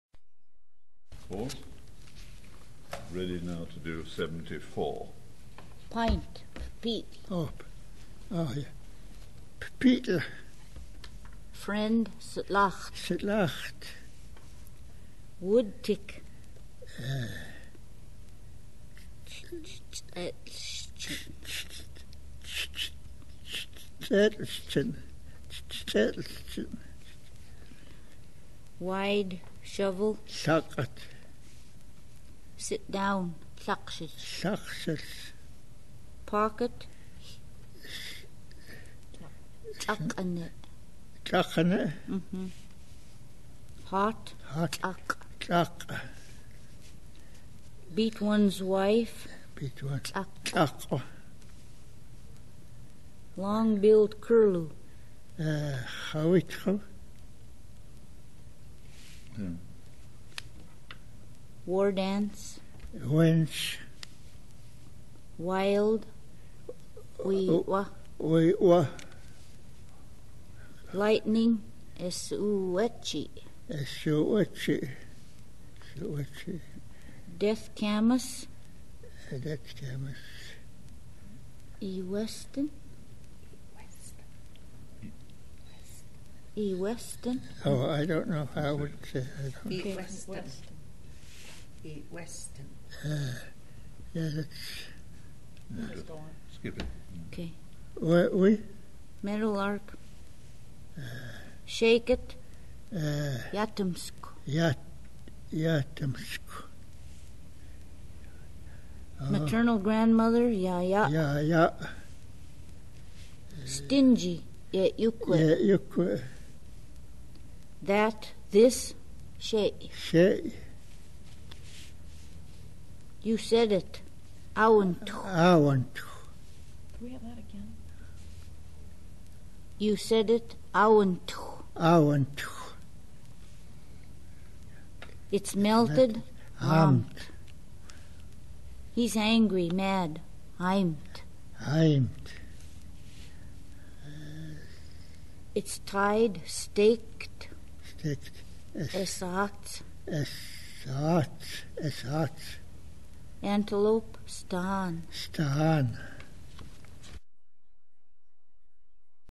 Flathead audio: fla_word-list_1992_21
Flathead (fla) audio: fla_word-list_1992_21
Language family: Salish; Interior Salish; Southern;
Flathead Cultural Center, Flathead Reservation, Montana
Date: 4/5/1992

Description: Language Montana Salish (Flathead).  fla_word-list_1992_01.html, enties: 70 - 92

Contributor: Dorothy Felsman, and her father, Joe Eneas
UCLA Phonetics Archive